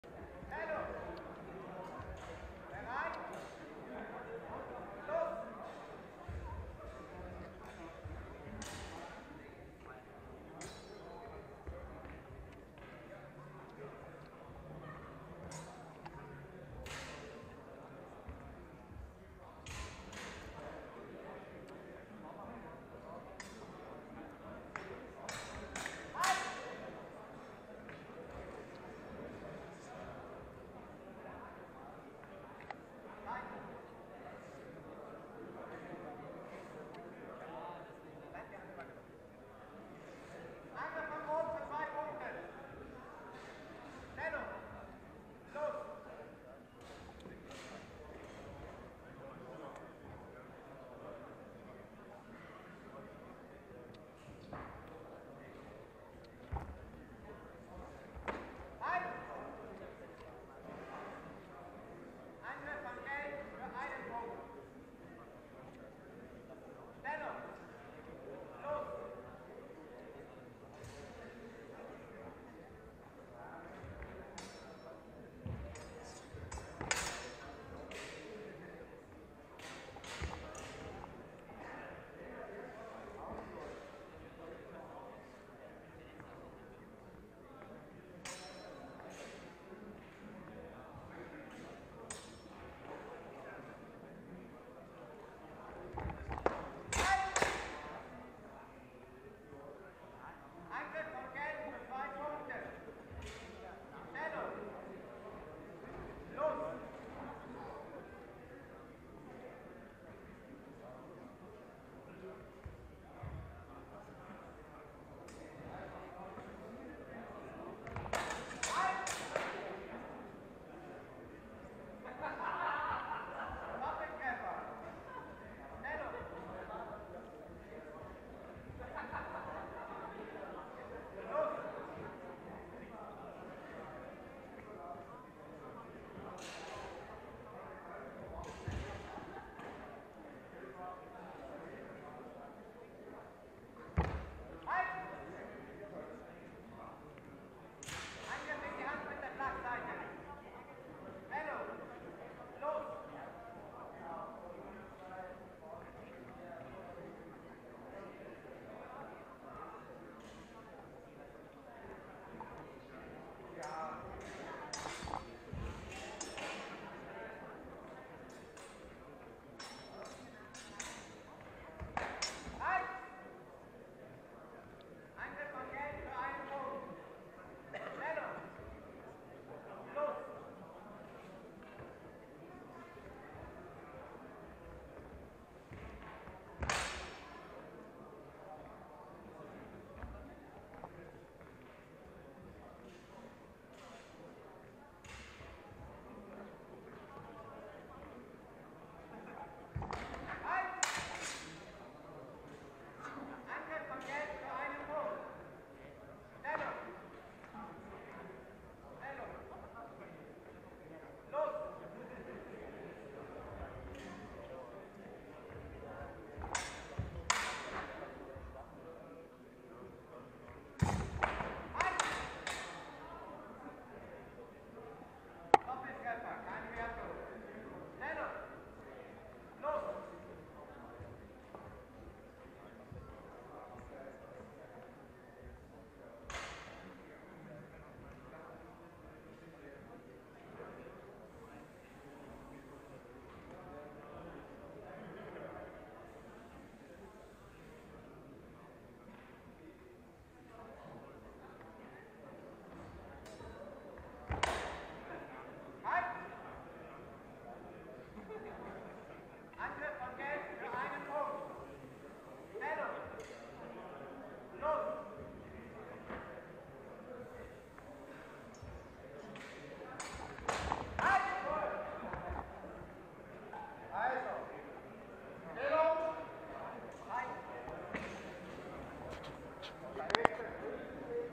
Swordtrip the Gathering 2018 - Semifinal - Alexander Fürgut vs. Lukas Mästle-Goer
Alexander Fürgut (red) vs. Lukas Mästle-Goer (yellow)
Win: Lukas